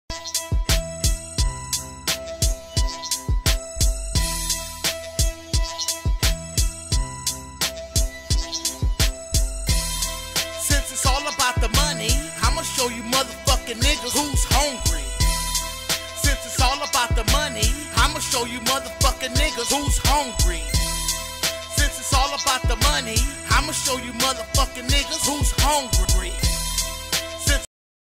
D.J. QUIK MONEY HUNGRY TOO
D.J. QUIK  A REAL COMPTON O.G. GETTING A MONEY HUNGRY SHIRT